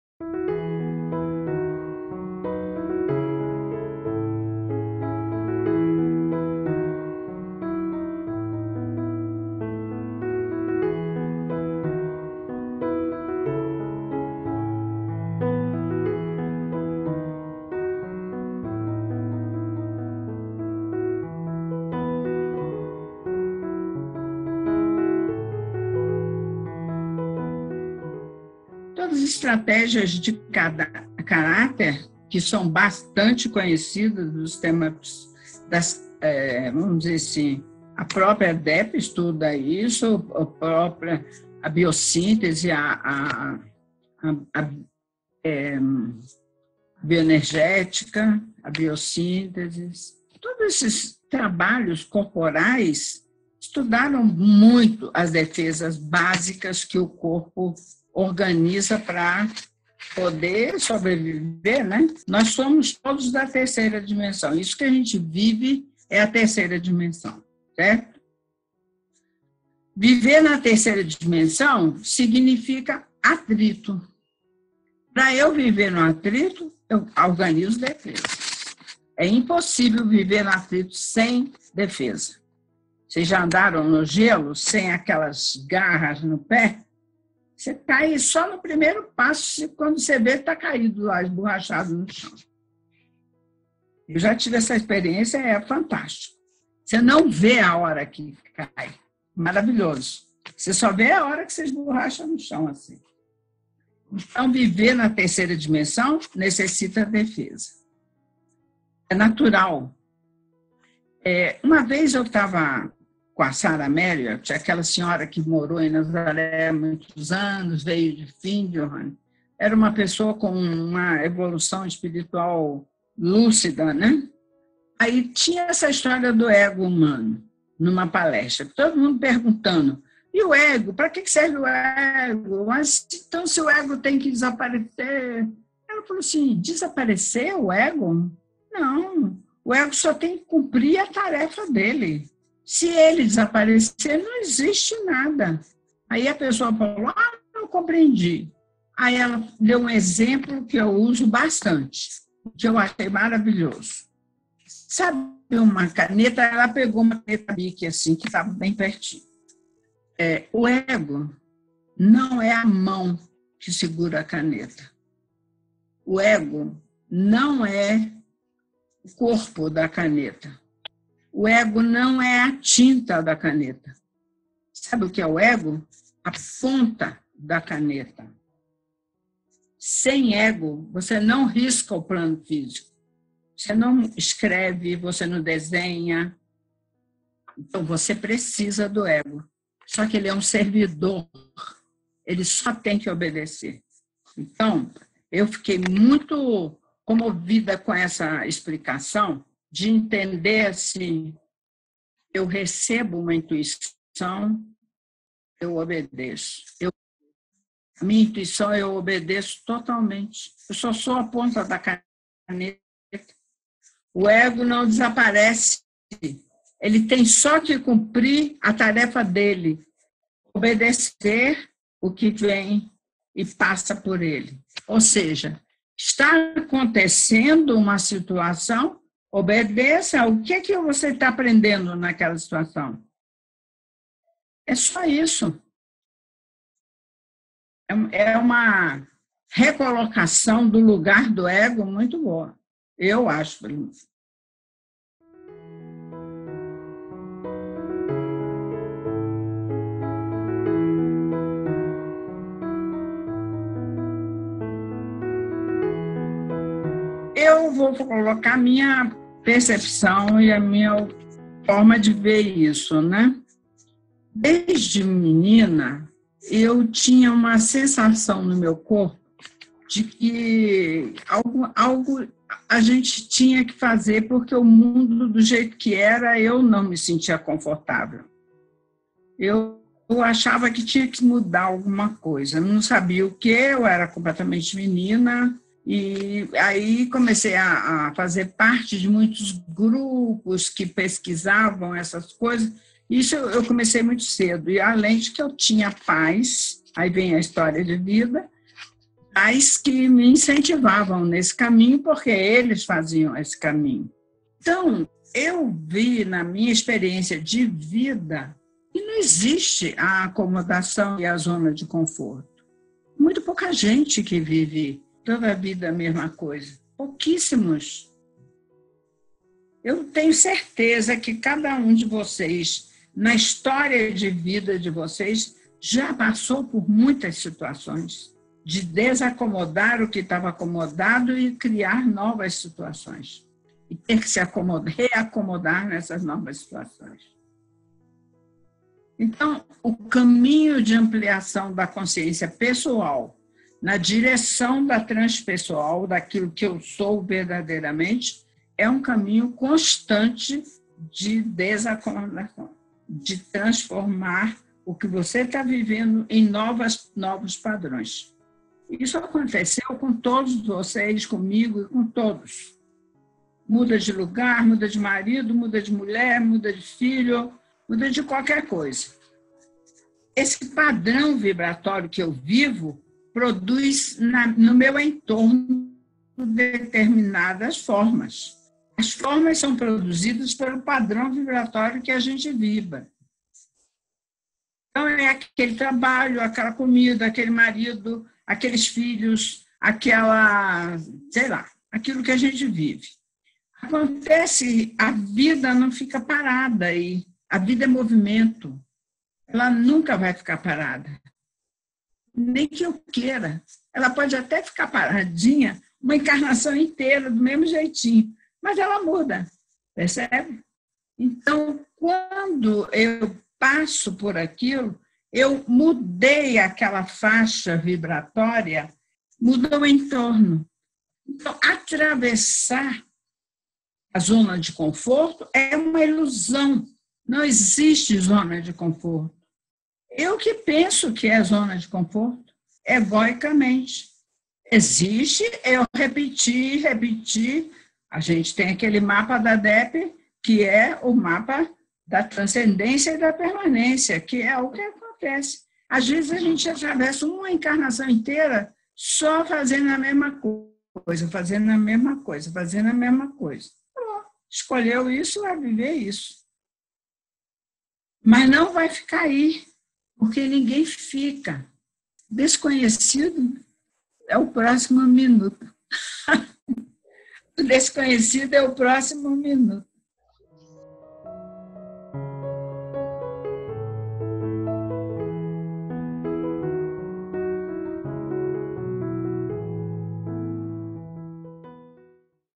Aidda Pustilnik – Desconhecido é o próximo minuto (Defesas Psíquicas e Zona de Conforto)
No vídeo desta semana, a psicóloga Aidda Pustilnik responde a novas perguntas realizadas no Encontro Magno, do programa Corporificando a Consciência, promovido pela Escola DEP, no dia 3 de outubro.

Neste encontro online, Aidda reflete sobre as defesas psíquicas de nosso corpo e porque é fundamental sairmos da zona de conforto.

Edição e Texto:
Gabriel Gama - @gabrielcngama | @paranaodesistir

ERRATA: Ao final do vídeo, foi exibido equivocadamente o Mapa da Espiral do Desenvolvimento da DEP, ao invés do Mapa da Transcendência e da Permanência.